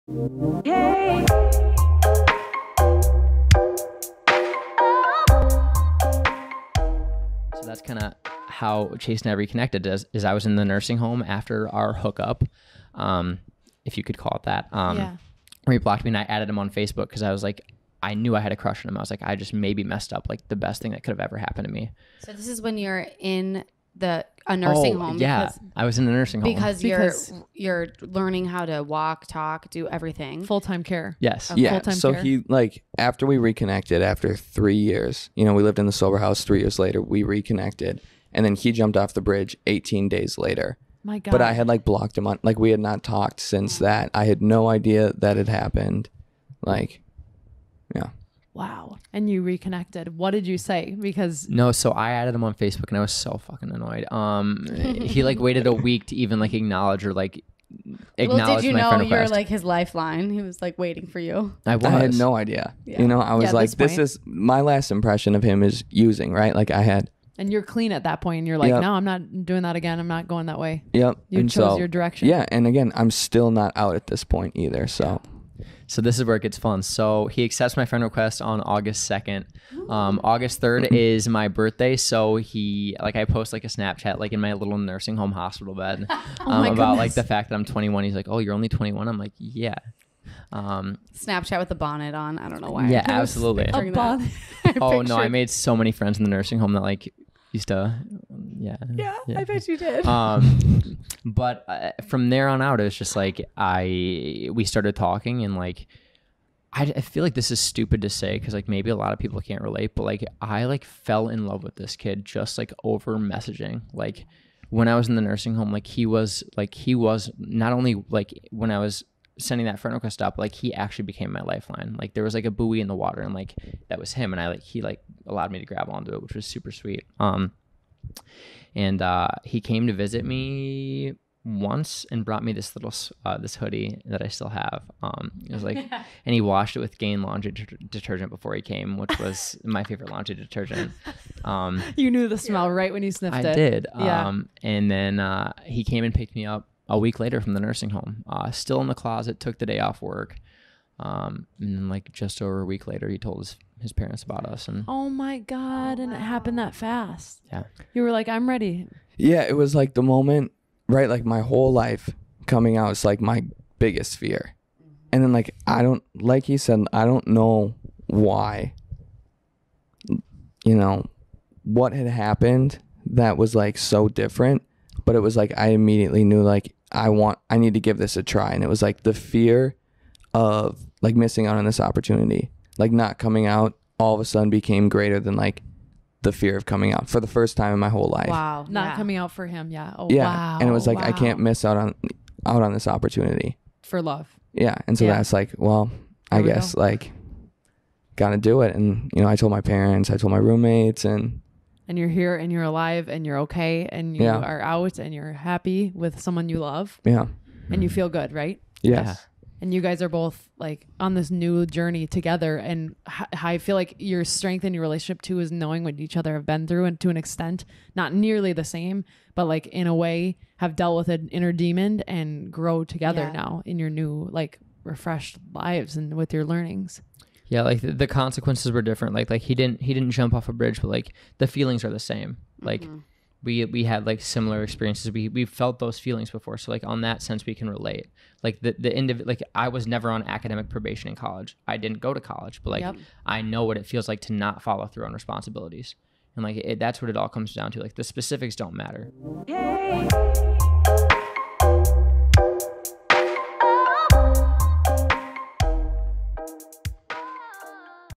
From Friends to Lovers
In this Straight Candid clip, Chase and John share how their relationship started along with Chase's coming out story to his parents.